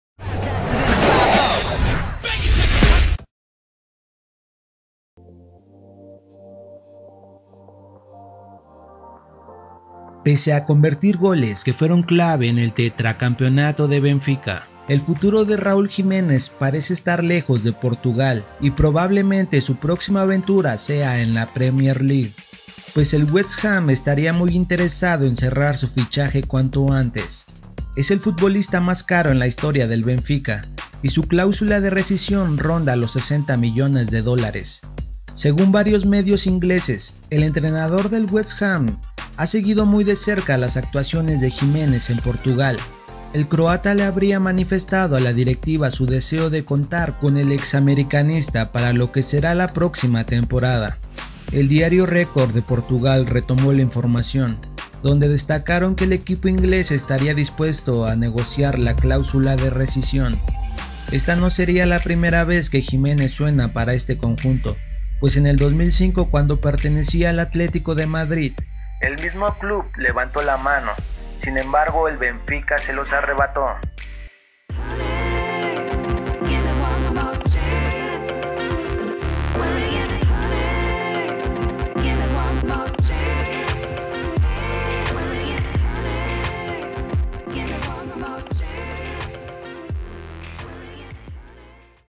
RAUL JIMENEZ cerca de la PREMIER LEAGUE ►► ESTE ES EL EQUIPO QUE LO PRETENDE
Raul Jimenez esta muy cerca de salir del benfica pues no son muchos los minutos disputados a pesar que marco goles importantes para que el benfica saliera campeon pero un equipo de la premier estaria dispuesto a tenerlo en sus filas , enterate de este Rumor.